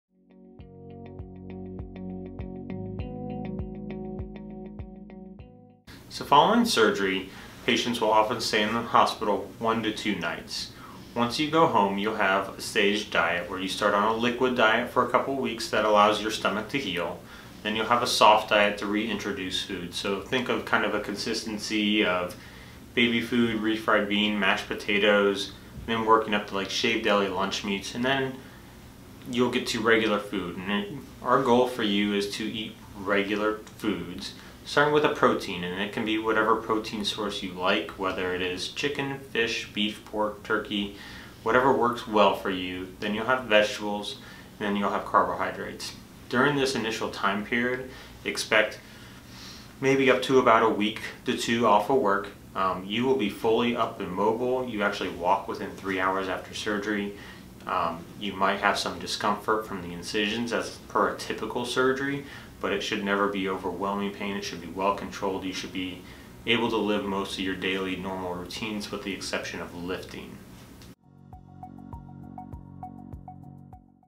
What to expect after bariatric surgery?
Jeff Henke, MD, a bariatric surgeon on staff at Medical City McKinney, explains what patients can expect following surgery.